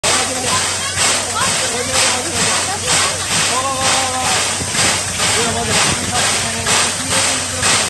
東武鉄道 SL大樹 C11‐207①停車中 2019.11.10
11月10日 14時53分頃 東武鉄道 下今市駅（栃木県日光市）にて 停車中の東武鉄道 SL大樹を撮影。
これから転車台に向かうため出力を上げているようです。

この日は前から気になって見たかった東武鉄道のSL 大樹を見に行くため、下今市駅（栃木県日光市）まで行きました。